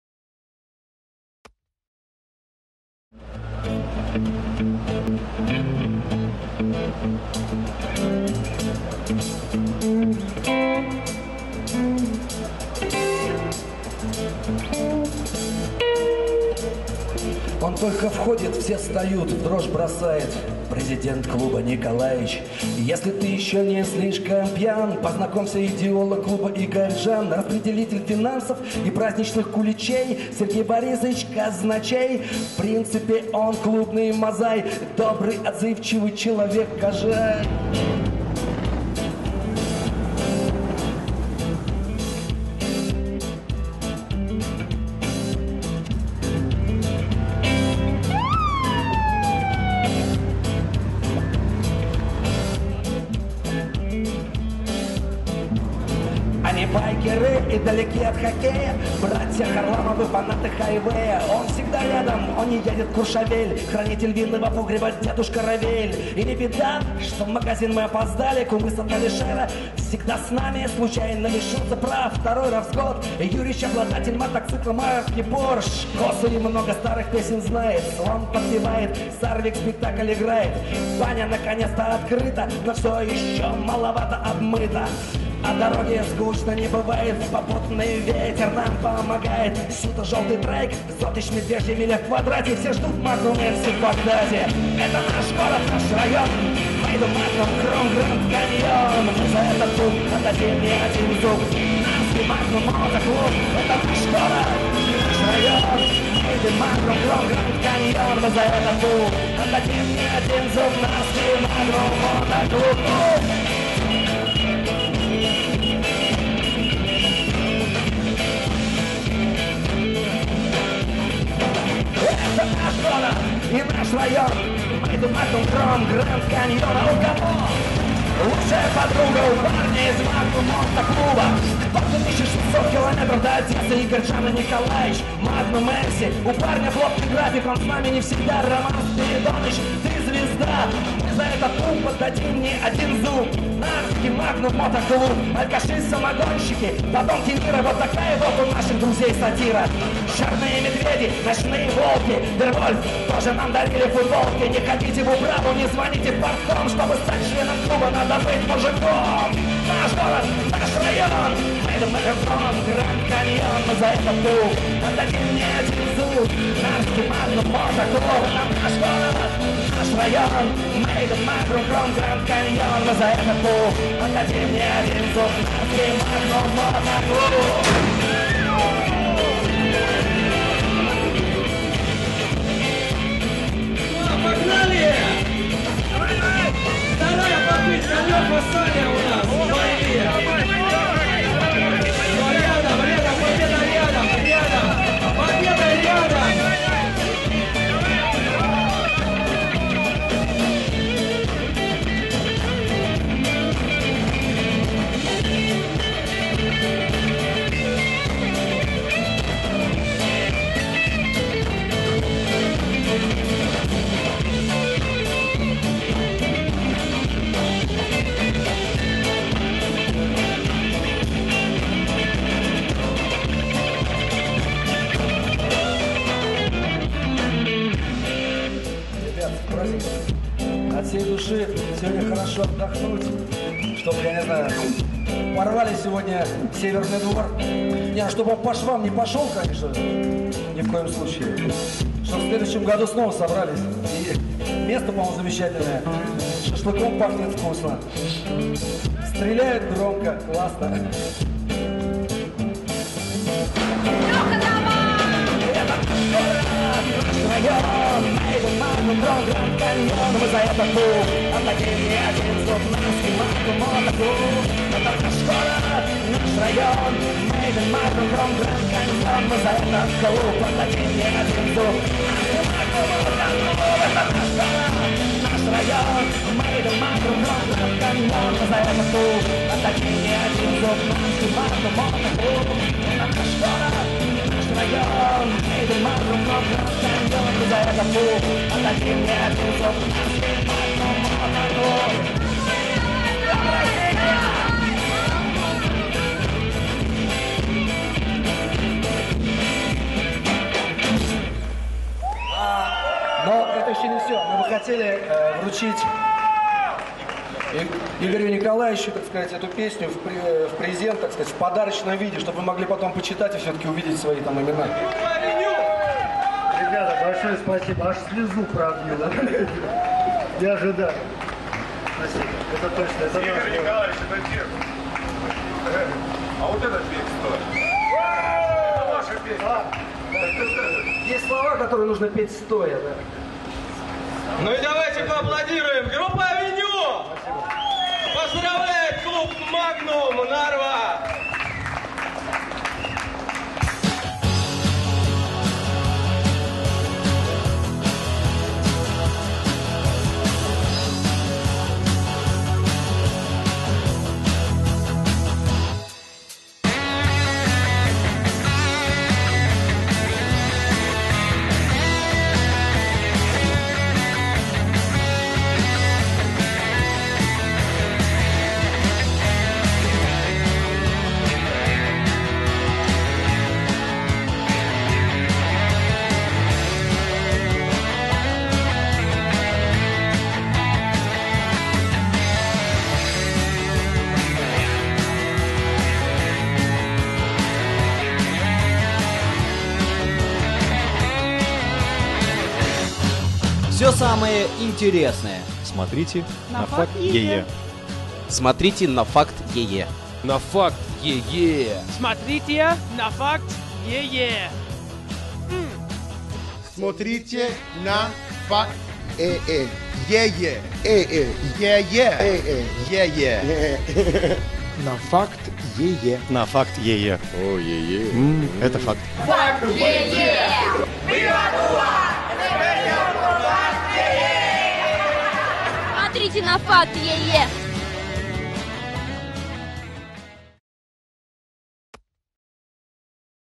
Группа AveNue, посвятила песню, мотоклубу Narva Magnum | Open Roads 2010
В 2010 году, на международном фестивале, Open Roads, который проходил в Нарве, группа AveNue, посветила песню, мотоклубу Narva Magnum. Предлагаем вашему вниманию, небольшую видео нарезку, с данного мероприятия, под песню группы AveNue.